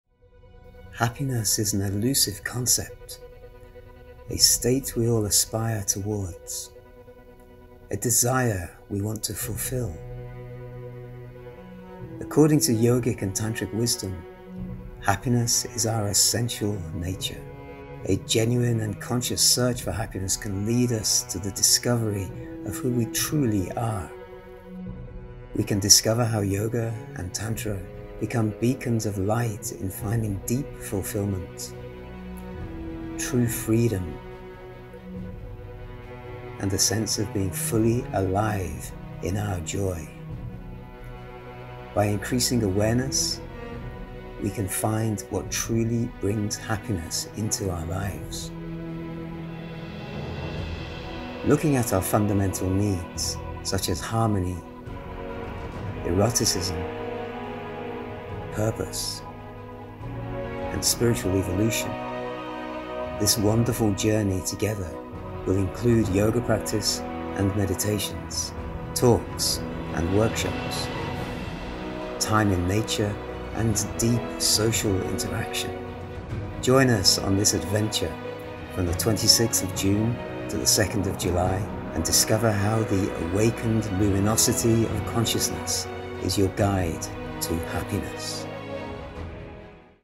Why join the Yoga & Happiness Summer Retreat?
The Summer Retreat is a wonderful opportunity for us to all to be together, learn and evolve together in a warm, friendly, nature filled ambiance.

Outside the big cities, in the nourishing green of Somerset, we can leave behind the stressors of daily life and offer ourselves a whole week of spiritual nourishment and soul awakening.

Every year the Summer Camp offers many revelations to those who participate. We will practice yoga together,  meditate, eat, laugh,  dance and participate in valuable workshops, seminars and experiences to quench our spiritual existence.

This years theme of Happiness is a subject that we all seek.  In this retreat, we will aim to spiritually nourish our souls through a deeper understanding of what Happiness is and how we can get there, so that we can all be long-lastingly happy profoundly in love with life itself.